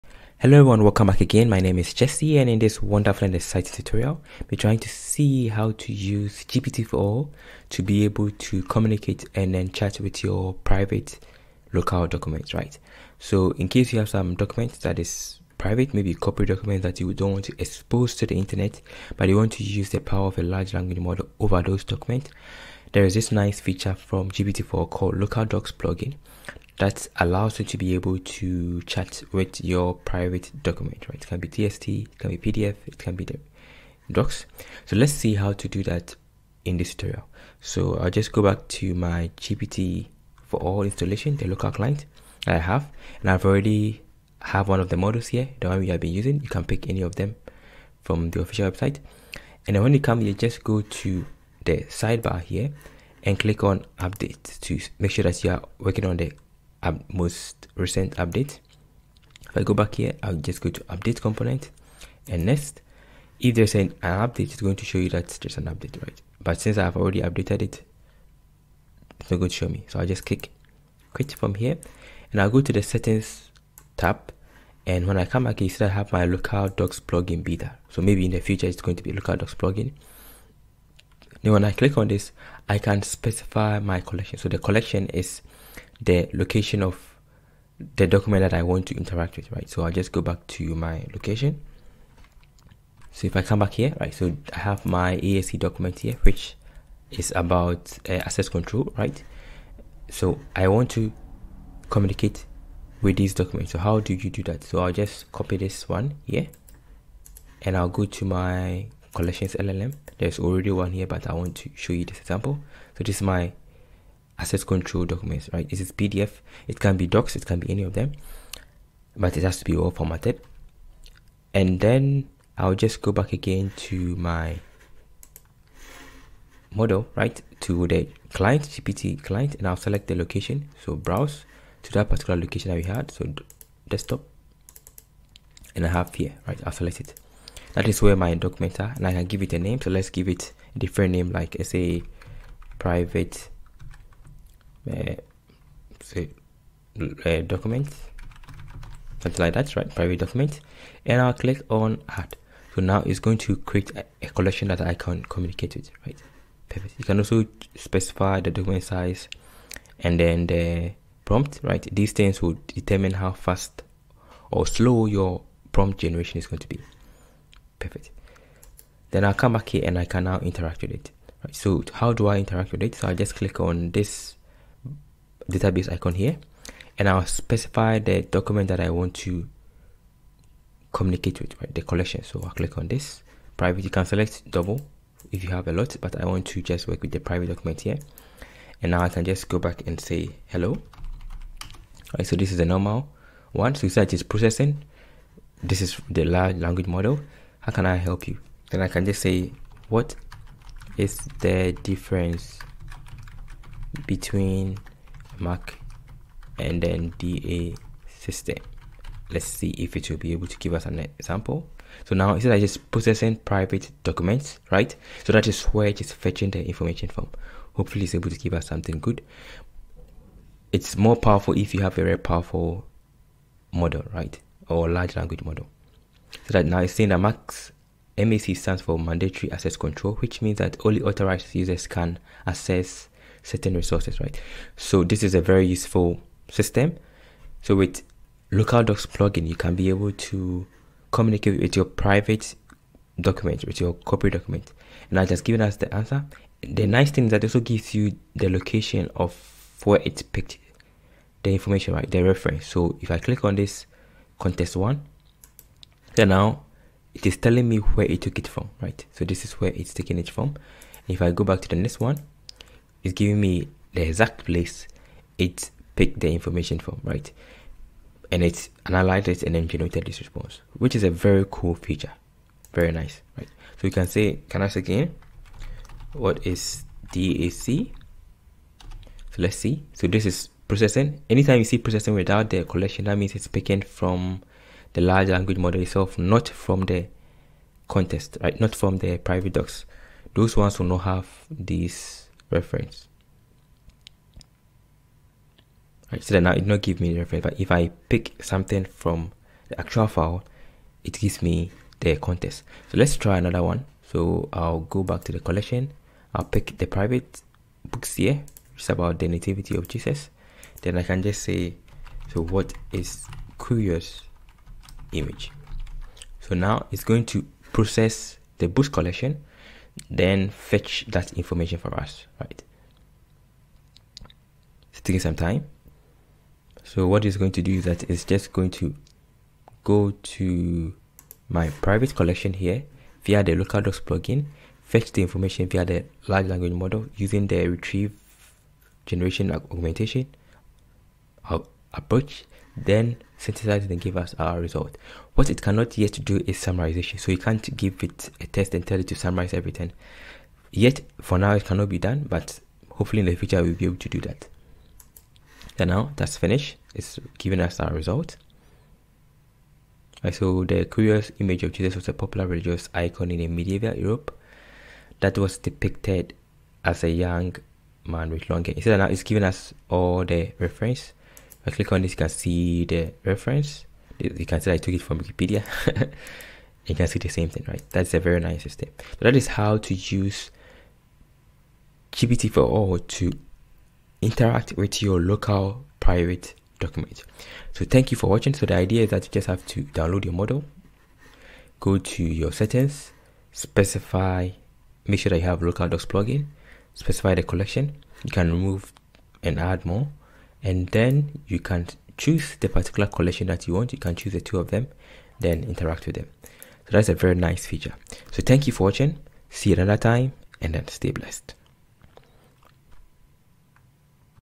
GPT4All - Chat with your Private Documents with LocalDocs Plugin
In this tutorial, we will explore  LocalDocs Plugin - a feature with GPT4All that allows you to chat with your private documents - eg pdf, txt, docx

⚡ GPT4All LocalDocs Plugin⚡ : Chat with your private documents

JCharisTech

⏲️===TimeStamps===⏲️
0:01 Introduction
00:53 Adding the LocalDocs Plugin
01:22 Collection of Private Documents
02:50 Interacting with Your Documents
04:05 Reference & Context
06:30 Recap